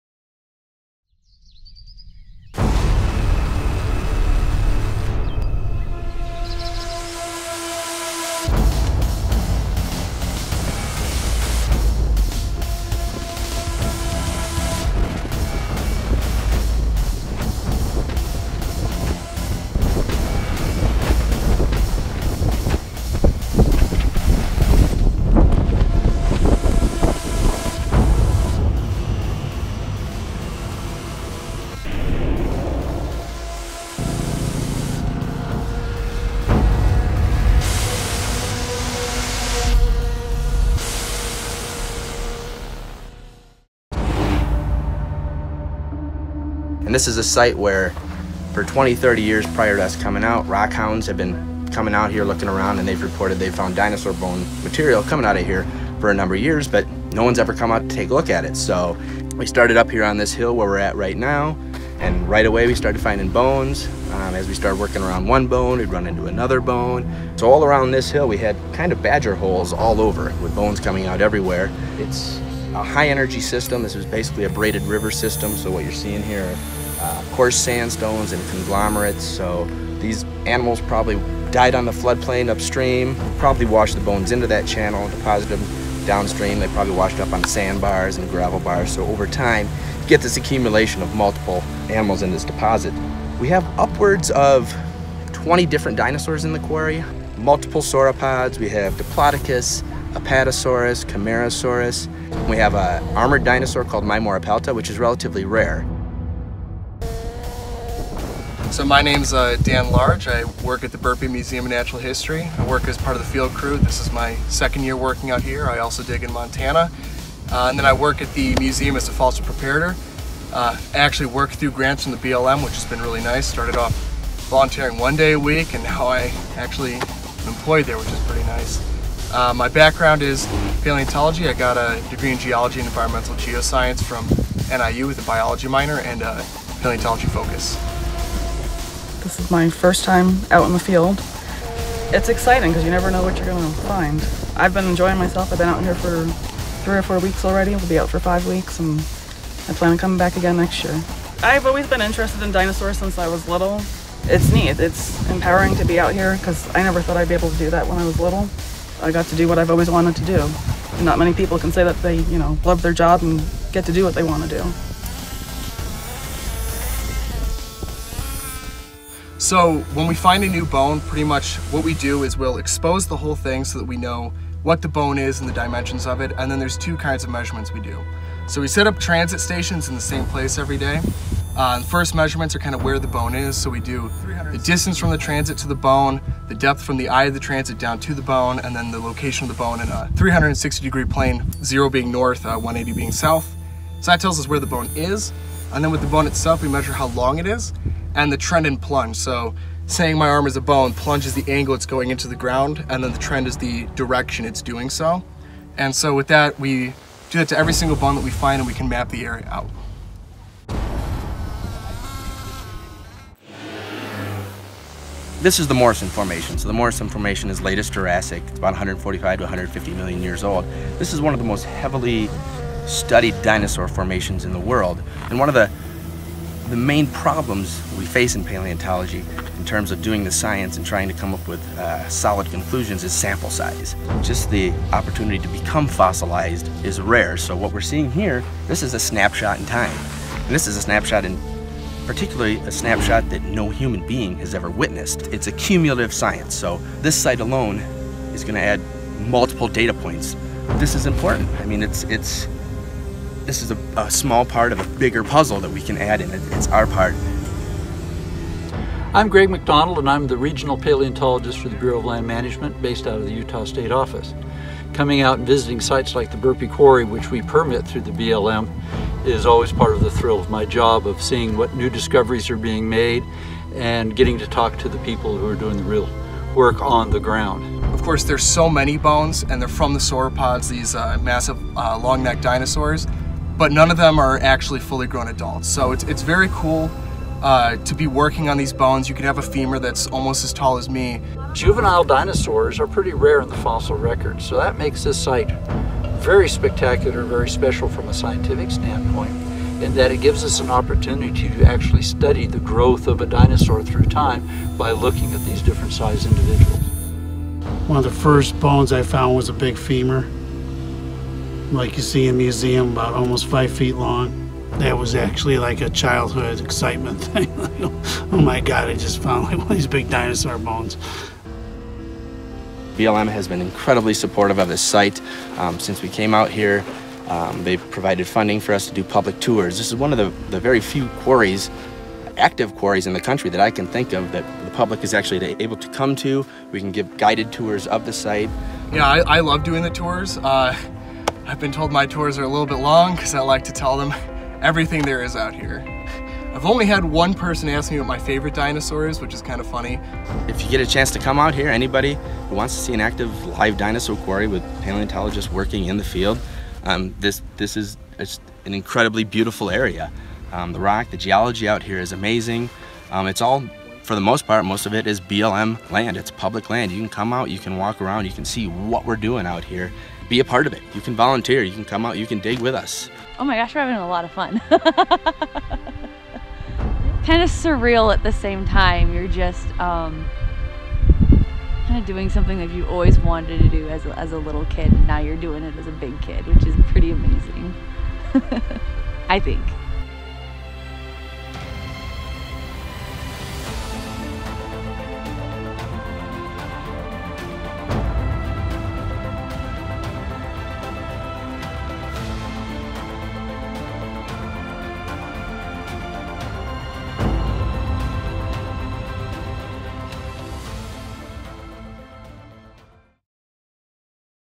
Hanksville-Burpee Dinosaur Quarry
An overview of the Hanksville-Burpee Dinosaur Quarry.  Thousands of years of dinosaur bones are scattered amongst the Morrison formation.  Join us to see live paleontological work, explore the area and appreciate the landscape.